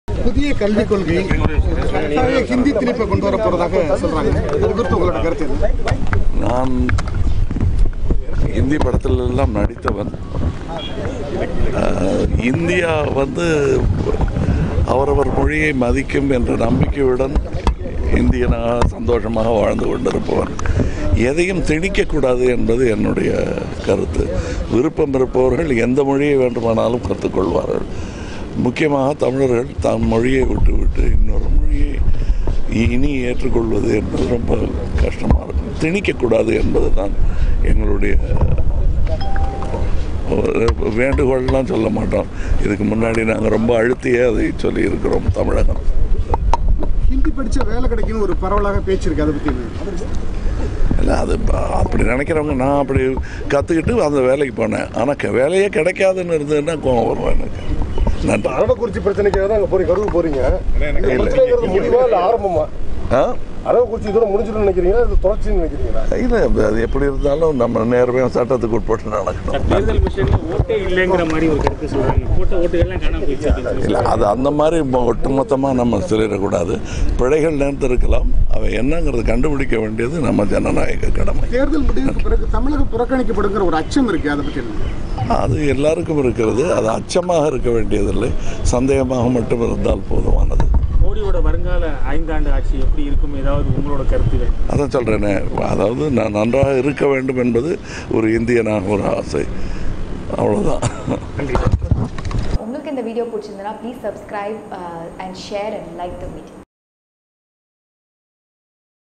இந்தி மட்டும் அல்ல எந்த மொழியையும் திணிக்கக்கூடாது கமலஹாசன்
இந்தி மொழி கட்டாயம் குறித்து கமலஹாசன் கருத்து kamal Speech About Hindi Compulsion Video On Cut2Cut.